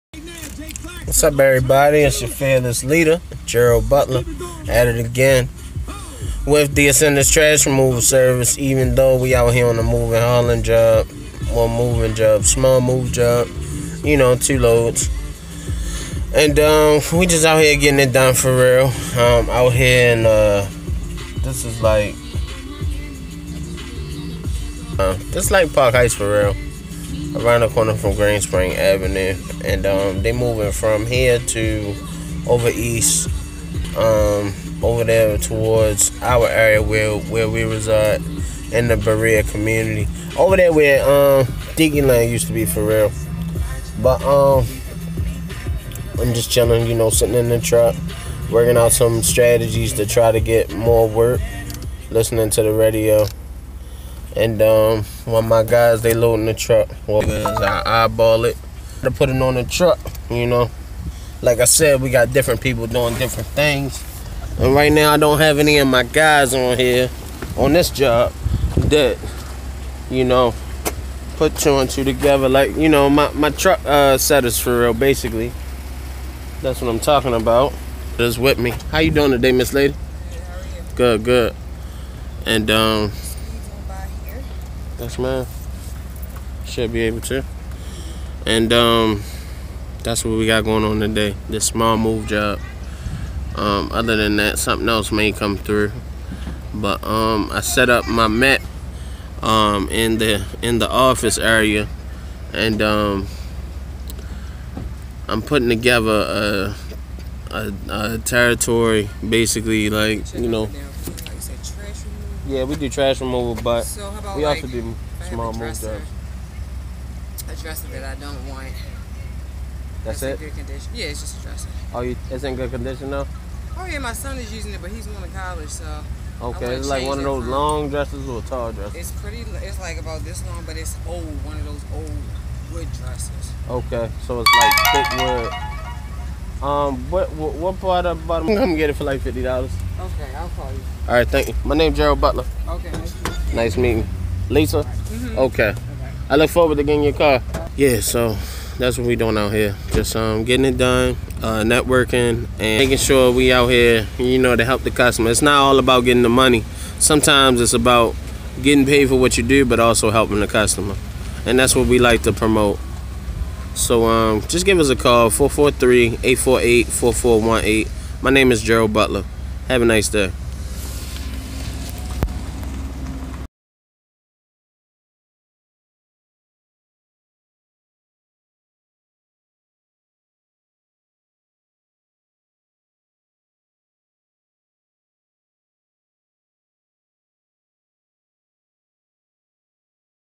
Tune in Tuesday
On a small move job, in West Baltimore near Park Heights. Also networking with the community around us.